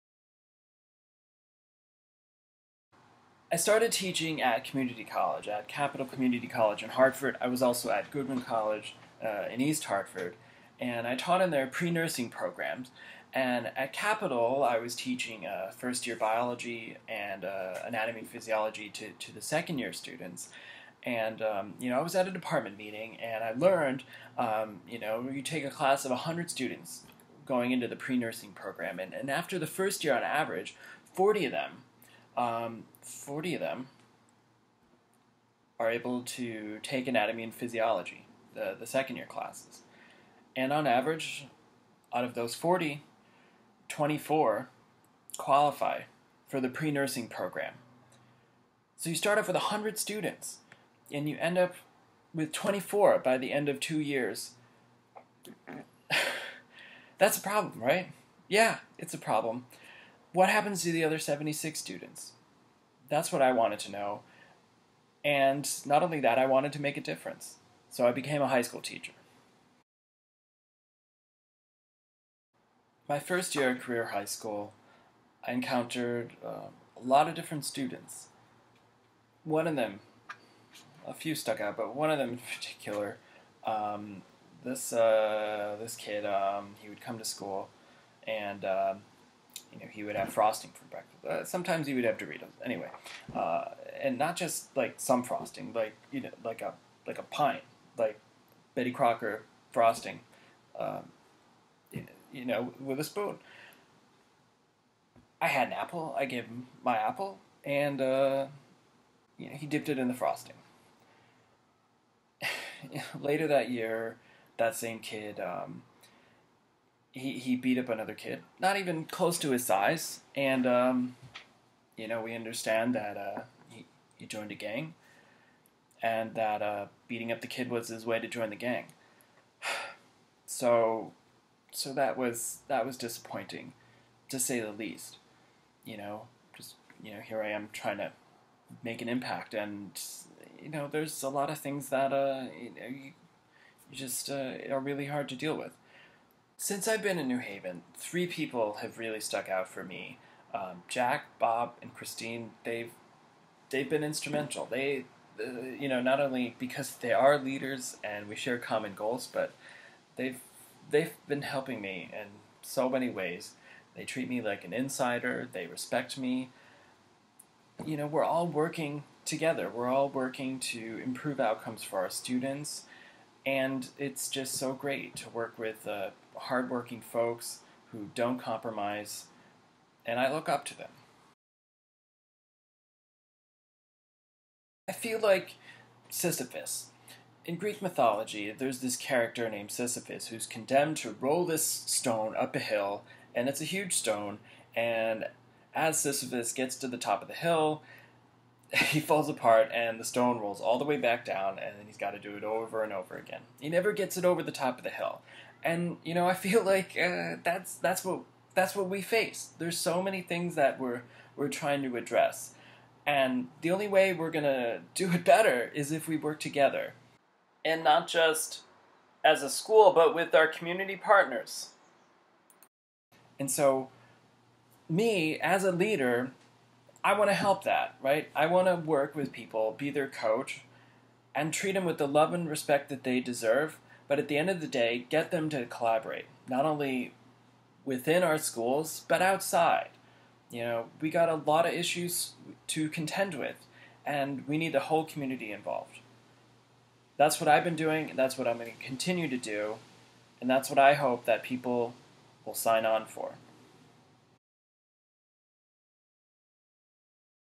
Becoming an Education Leader
I put together this video as part of my preparation ahead of staring an Administrator Preparation program at UCONN's NEAG School of Education with several fellow New Haven educators, which I'm starting in July '16!
Why I am working to become an education leader, who inspires me, and what type of leader I hope to be known as!